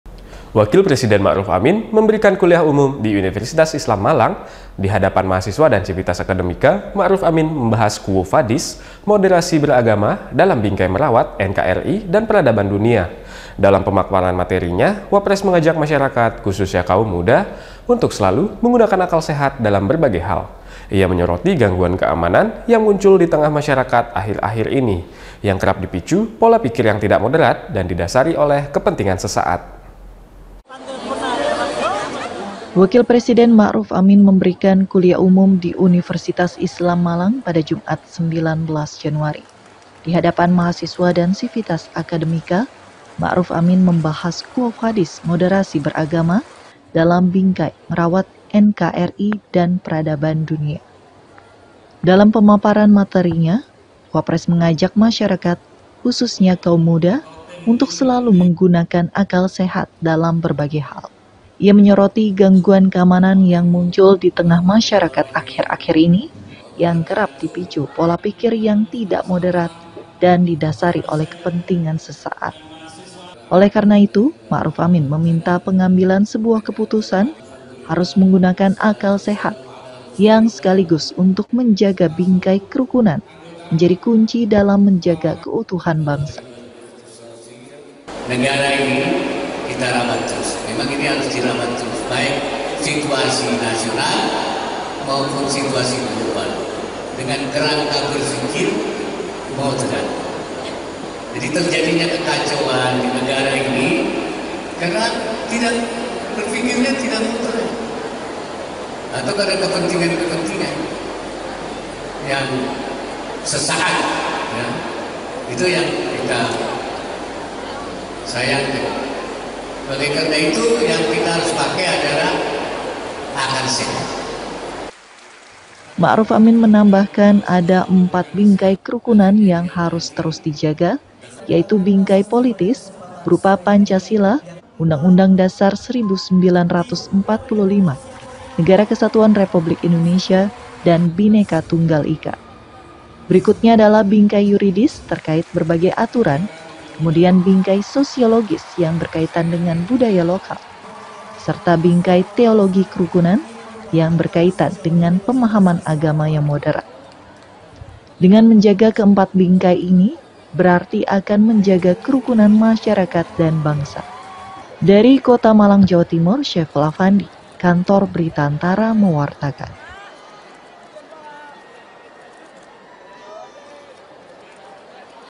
WAPRES MA’RUF AMIN AJAK BERPIKIR MODERAT DAN GUNAKAN AKAL SEHAT || BERANDA NUSANTARA
Terjadinya berbagai gangguan keamanan atau kekacauan selama ini salah satunya
dipicu oleh pola pikir masyarakat yang tidak moderat dan didasari kepentingan
sesaat. Maka dari itu dalam mengambil sebuah keputusan harus menggunakan
akal sehat. Selain itu, menjaga bingkai kerukunan menjadi kunci dalam menjaga
keutuhan bangsa ini. Hal itu yang disampaikan wakil presiden RI, Ma'ruf Amin saat
mengisi kuliah tamu di Unisma.